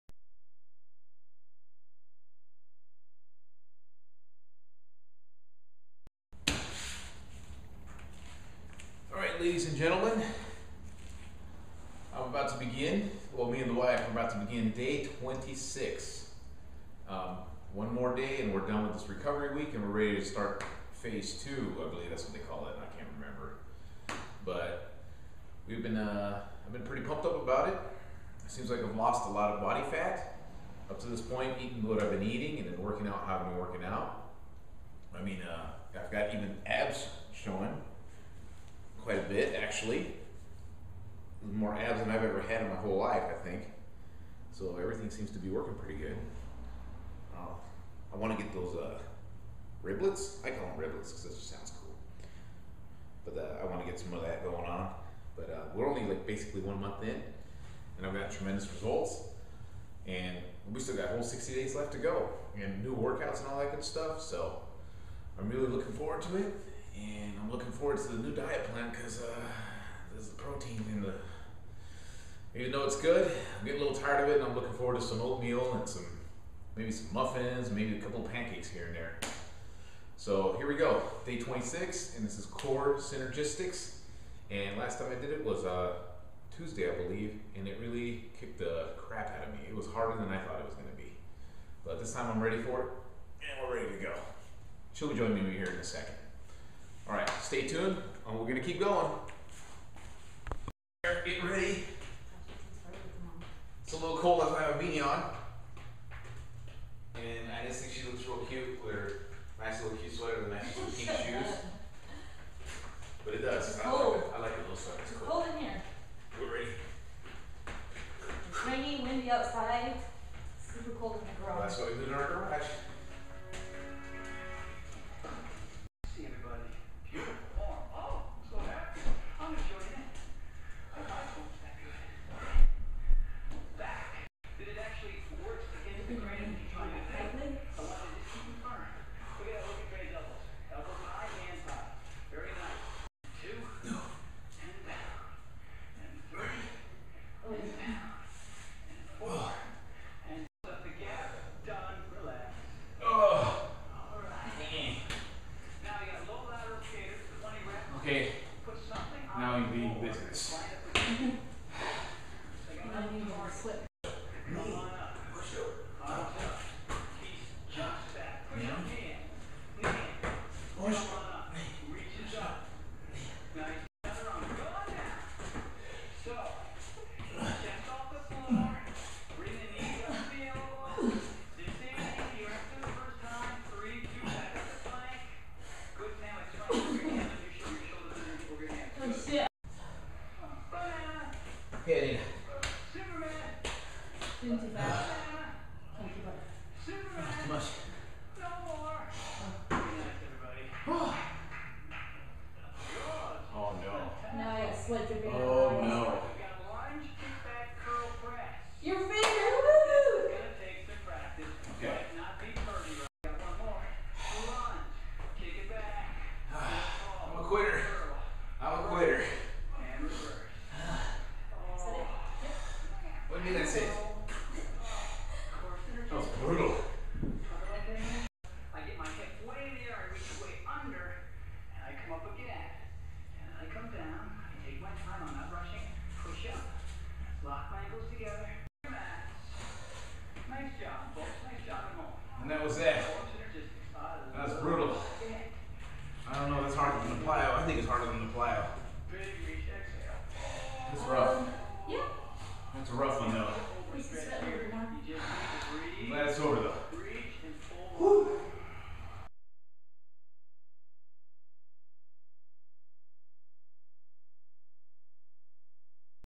P90X Day 26 of all 90 - Core Synergystics
One more round of core synergystics. Wow this one is tough!